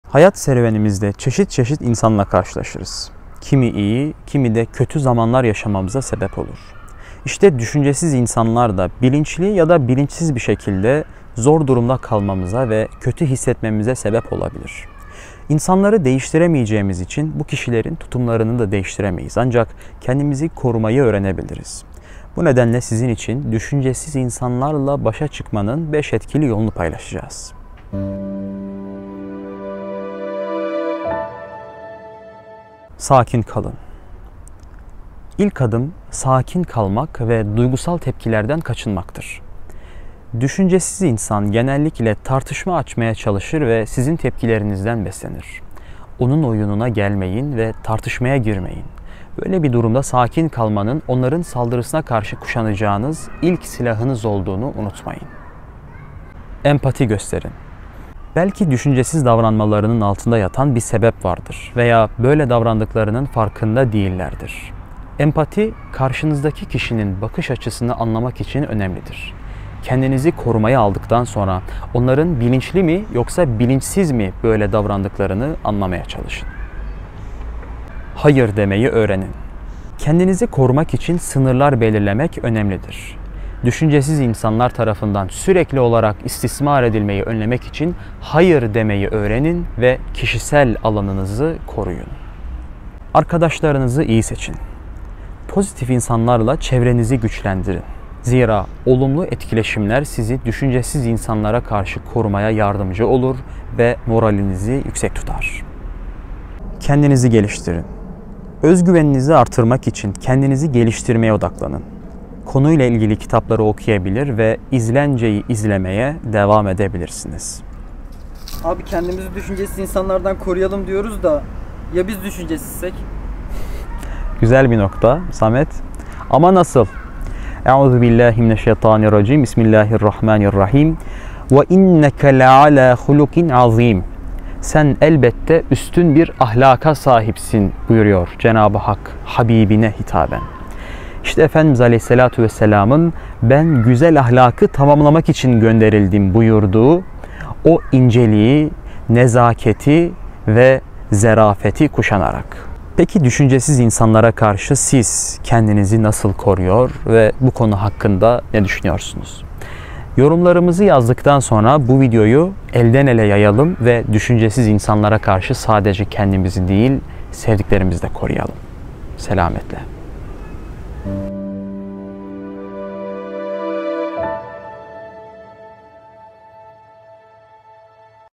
Düşüncesiz İnsanlara Karşı Kendimi Nasıl Koruyabilirim?
Hayat serüvenimizde çeşit çeşit insanla karşılaşırız. Kiminin yanında iyi hissederken kimi de sıkıntılı zamanlar yaşamamıza neden olur. Düşüncesiz insanları değiştiremeyiz ancak kendimizi korumayı öğrenebiliriz.

Bu videoda düşüncesiz insanlarla başa çıkma yollarından bahsedeceğiz.

Peki siz düşüncesiz insanlarla karşı karşıya kaldığınızda ne yaparsınız?